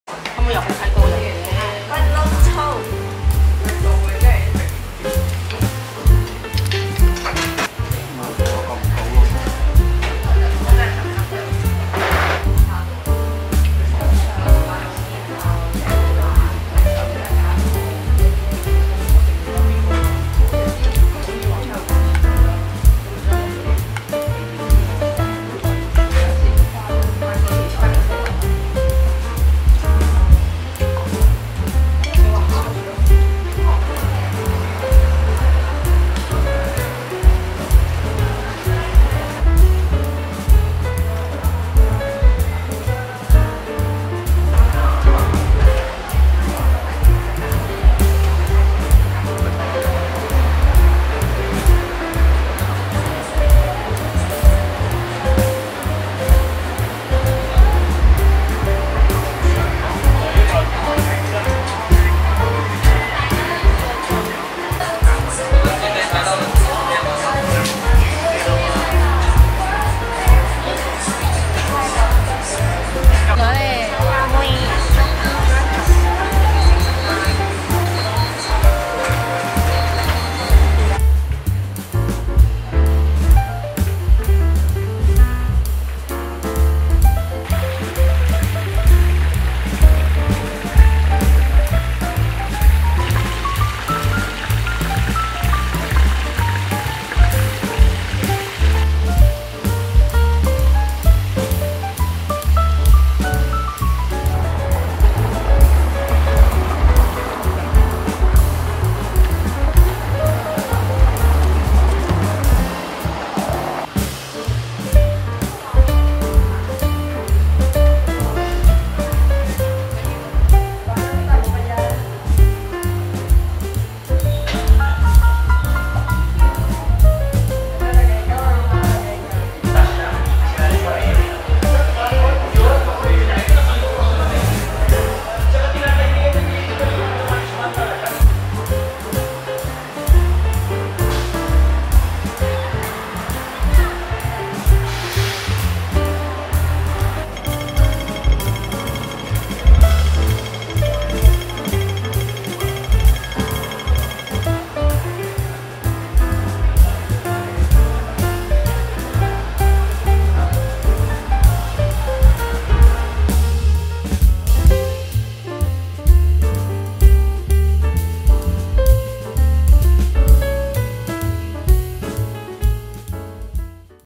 a week in macau / 1st day
7 days 7 nights in Macau & 1 day in Hong Kong.
This is the first day vlog in Macau. We arrived there at about 9:30pm, felt tired and hungry. After dropping off luggage at the hotel, we went to a restaurant for supper. Everyone misses the food here😂！
And we highly recommend a must-eat snack,  please watch the video😋.
Enjoying all the food there, walking around historical sites at this former portuguese colony.
More macau vlogs are coming soon.

🎬 previous hong kong🇭🇰 & macao🇲🇴 vlog!